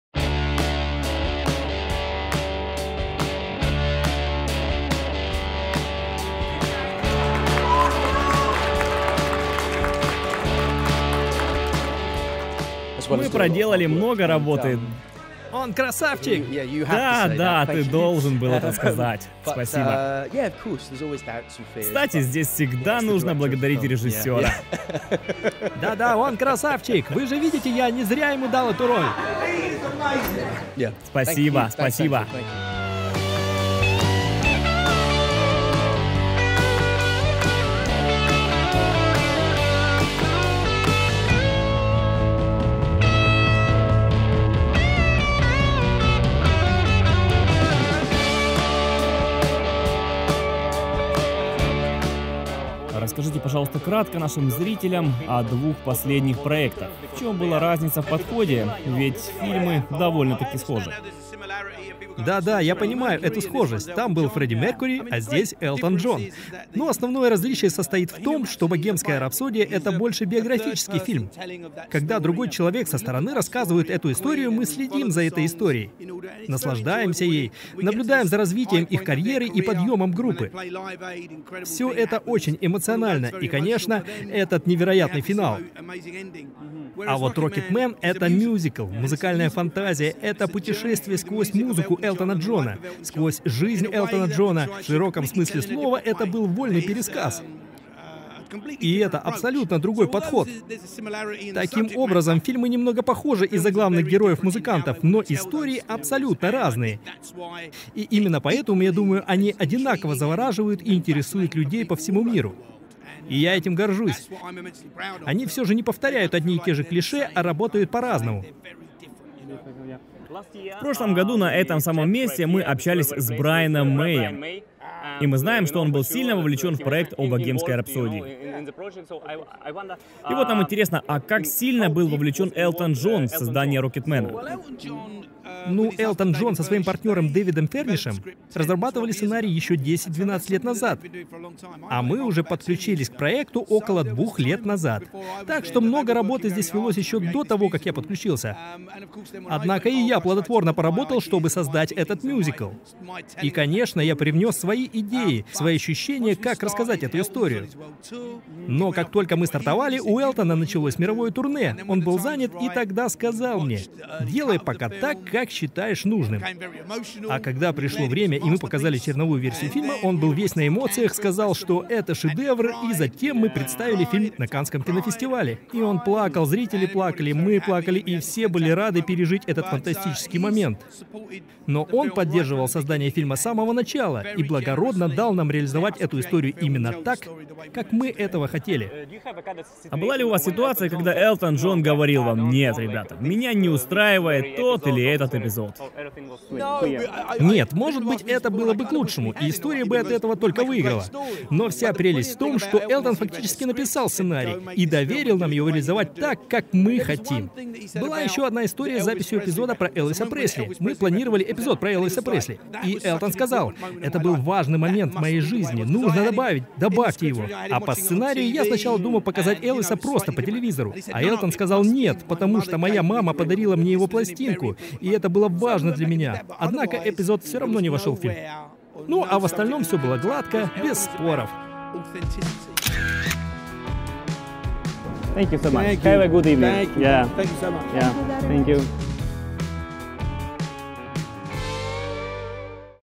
Декстер Флетчер: Интервью. Как Режиссер Снимал Богемскую Рапсодию и Рокетмена?
Интервью daynight.tv - Декстер Флетчер: режиссер Рокетмена и Богемской Рапсодии